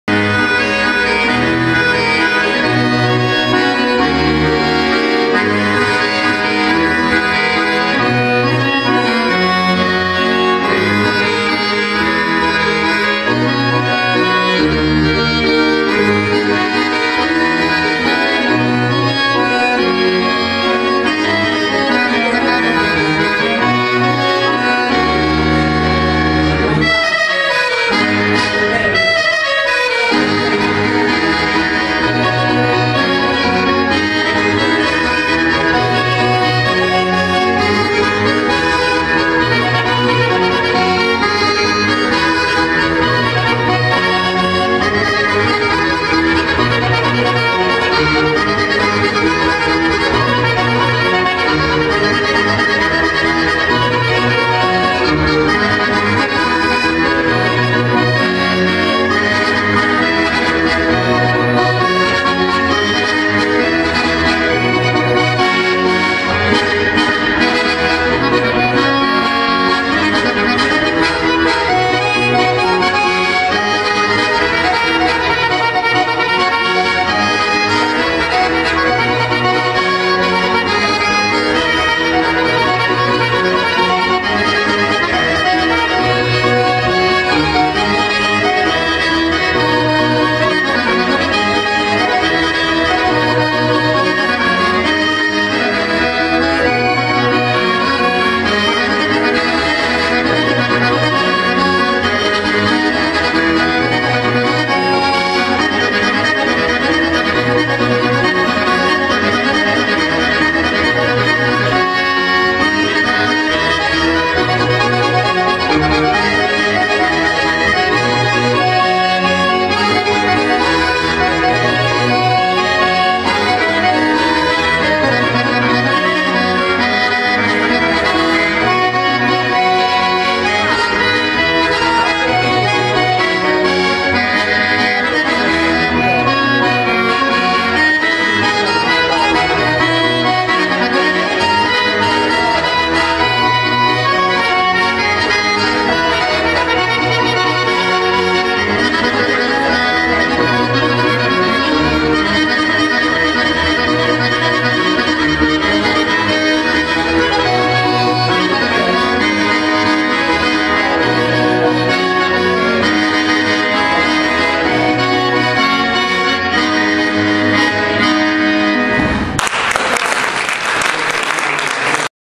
Pietro Porcu di lula e Pierpaolo Vacca di Ovodda Virtuosismi
Una bellissima suonata del "duo" composto da Pietro Porcu di Lula e Pierpaolo Vacca di Ovodda, venuti a trovarci in una delle serate di "A Ballare a Ballu Sardu" di Tonino Bonu